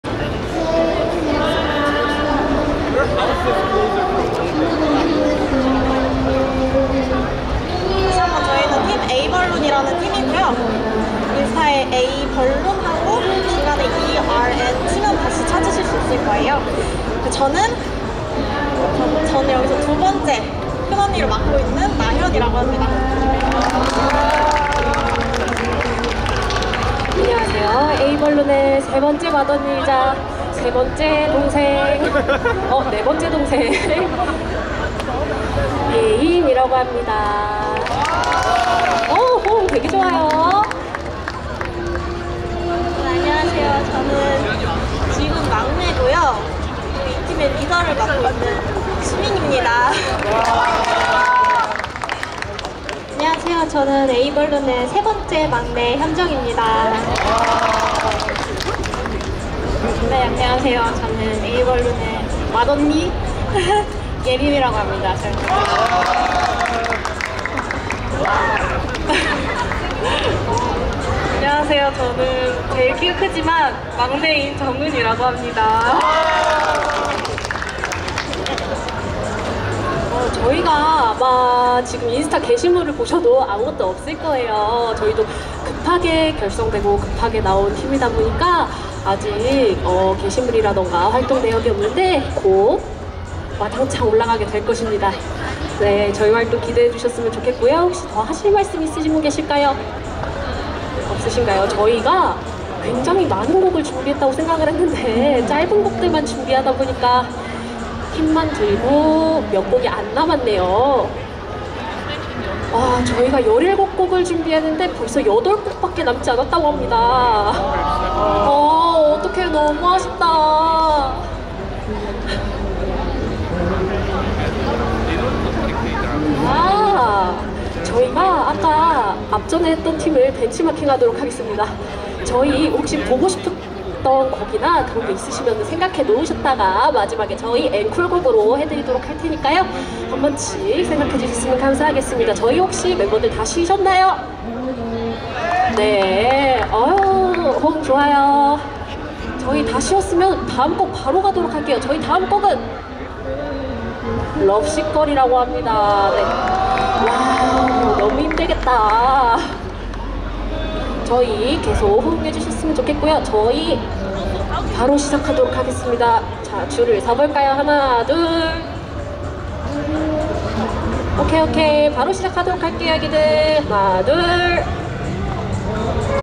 차세대 춤꾼 에이벌룬 - 팀 소개 개인소개 .
신생 댄스팀 에이벌론 -  차세대 춤꾼 동아리발표회 .  신촌 연세로 스타광장 버스킹 .이 영상은 홍낙현의 제작 영상이며, 홍낙현에 의해 촬영, 편집되었습니다.